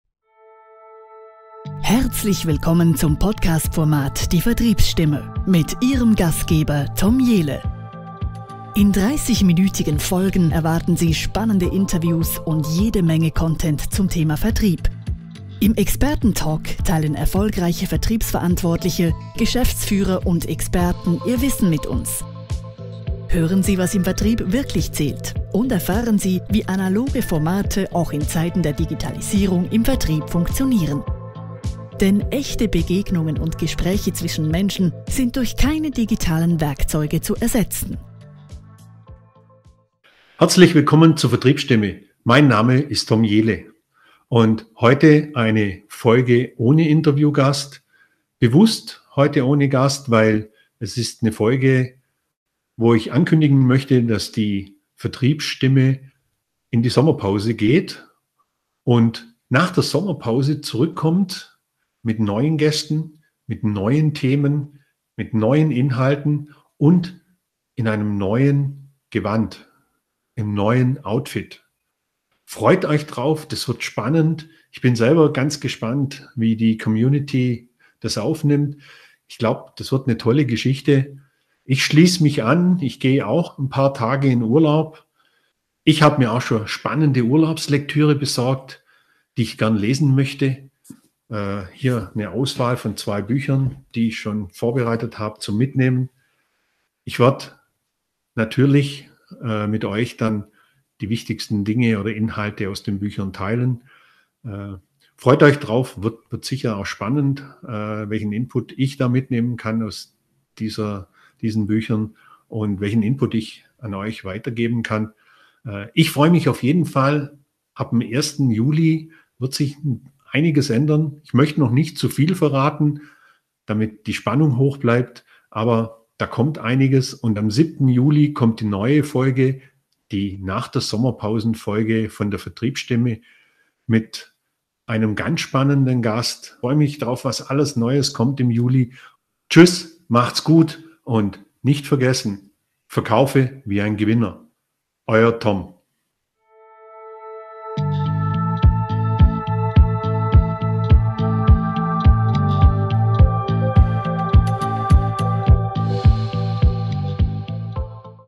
Die VertriebsStimme - Ab in die Sommerpause 2021 - Folge 19
Die VertriebsStimme - geht in die Sommerpause.
Nach 19 Folgen gönnt sich die VertriebsStimme eine kurze Auszeit, um am 7 Juli 2021 frisch gestärkt mit neuen tollen Gästen, viel Input zum Thema Vertrieb und interessanten Geschichten, wiederzukommen.

Die VertriebsStimme bekommt in der Sommerpause auch ein neues Gewand, wir haben frisch gestrichen, neu gefliest, naß durchgewischt und alles schön gemacht. Seit gespannt auf Folge 20.

Unbedingt am 7 Juli 2021 reinhören.
Die VertriebsStimme by Tom Jehle